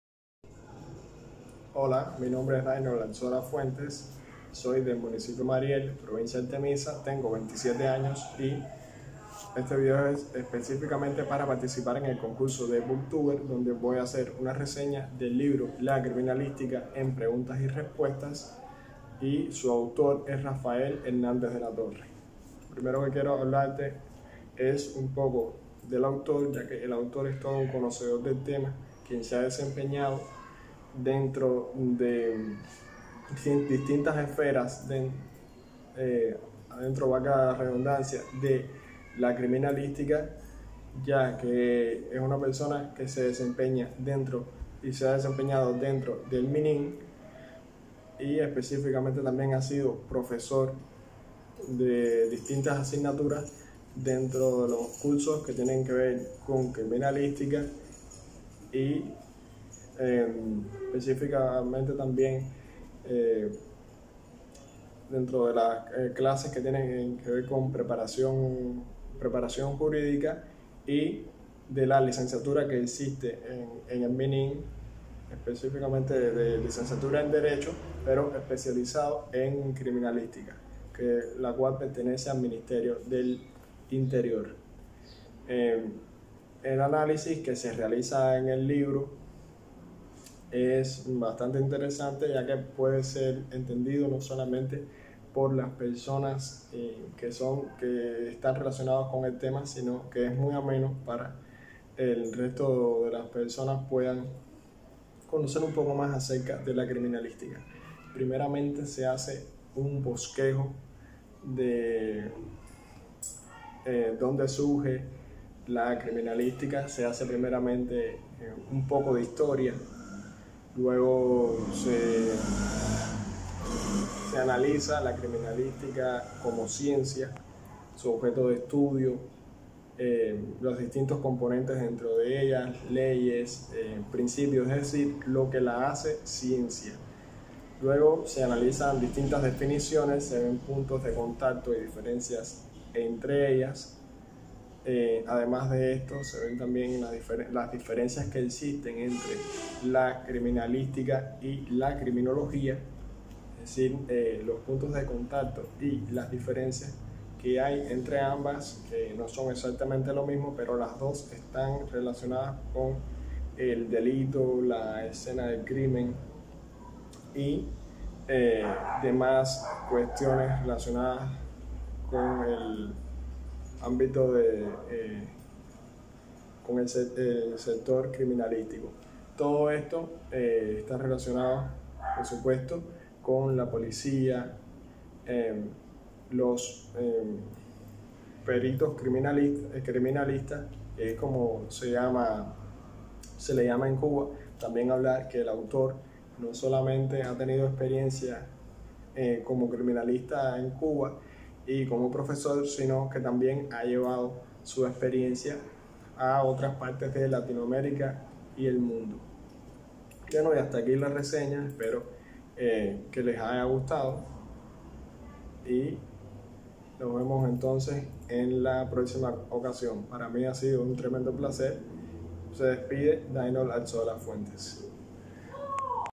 Daynol Arzola Fuentes- Concurso Booktuber
Mi nombre es Daynol Arzola Fuentes del municipio Mariel, provincia Artemisa. Tengo 27 años de edad y la reseña que les traigo es de él libro La Criminalística en Preguntas y Respuestas que tiene como autor a Rafael Hernández de la Torre